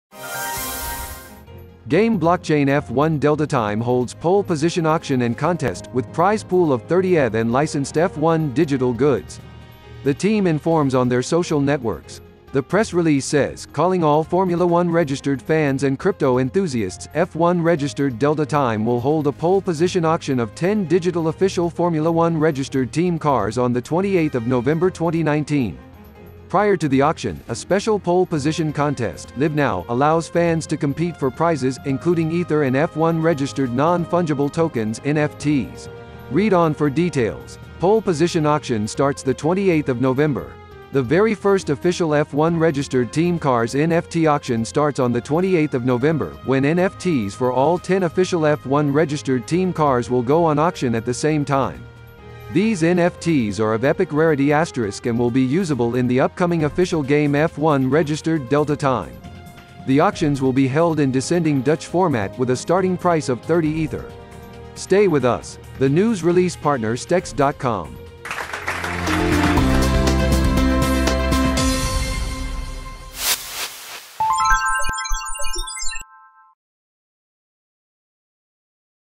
KCN F1® DELTA TIME - POLE POSITION AUCTION & CONTEST
Game Blockchain F1DeltaTime holds pole position auction and contest, with prize pool of 30 ETH and licensed F1 digital goods! The team informs on their social networks. The press release says: “Calling all Formula1® fans and crypto enthusiasts: F1® Delta Time will hold a Pole Position Auction of 10 digital official Formula 1® team cars on 28 November 2019. Prior to the auction, a special Pole Position Contest (live now) allows fans to compete for prizes, including ether and F1® non-fungible tokens (NFTs). Read on for details! Pole Position Auction starts 28 November. The very first official F1® team cars NFT auction starts on 28 November, when NFTs for all 10 official F1® team cars will go on auction at the same time. These NFTs are of Epic rarity* and will be usable in the upcoming official game F1® Delta Time. The auctions will be held in descending Dutch format, with a starting price of 30 ether”. Stay with us! The news release partner STEX.com.